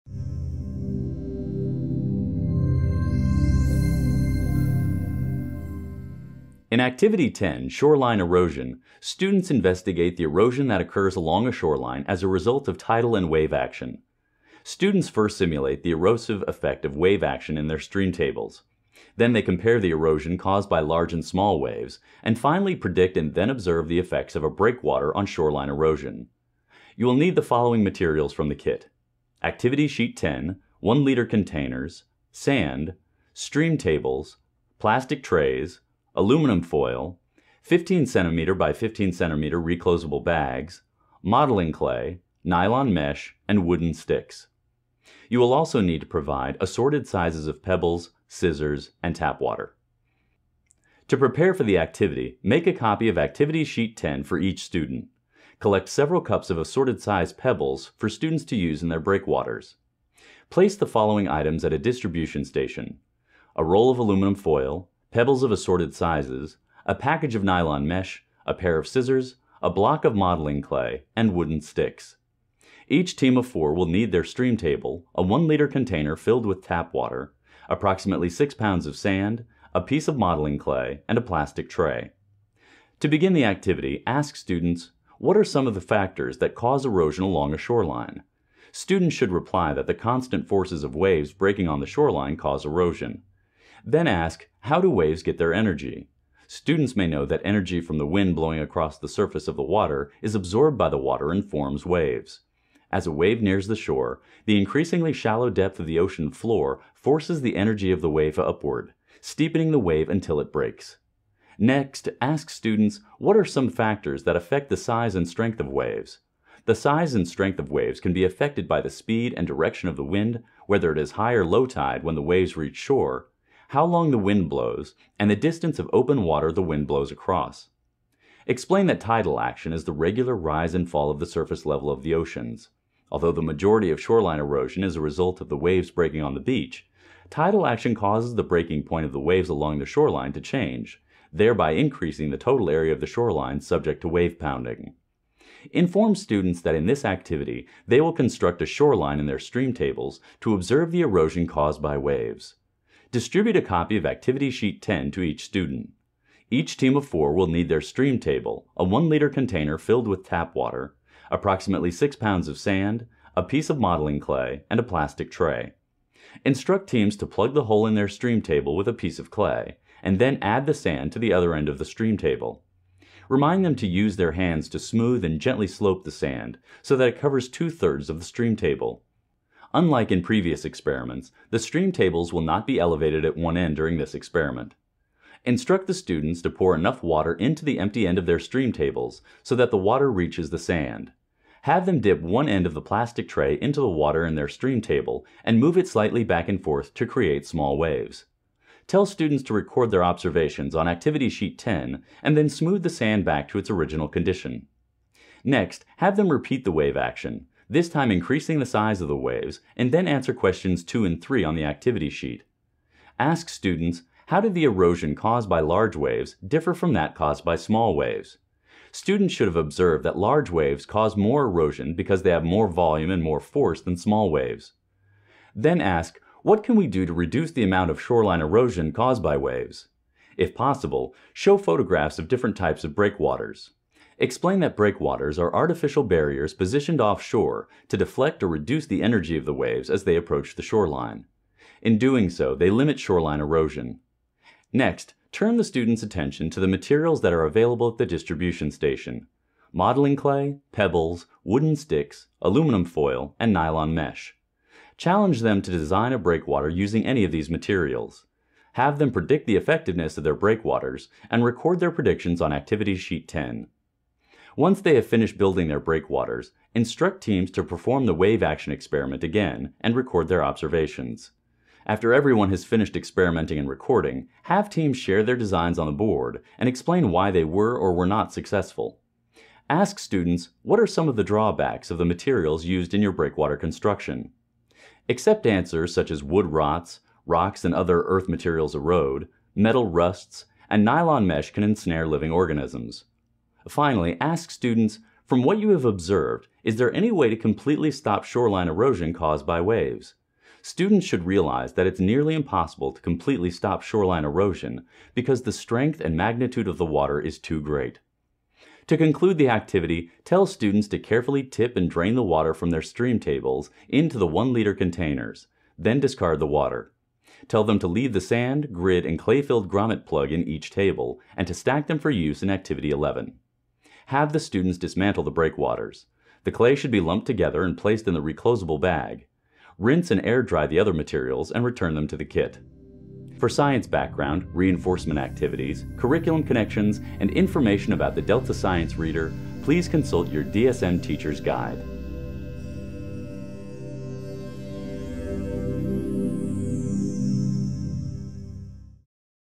Erosion - Activity 10: Shoreline Erosion
Stream tables become beaches as students investigate shoreline erosion caused by waves. They design and construct breakwaters and evaluate their effectiveness in reducing shoreline erosion.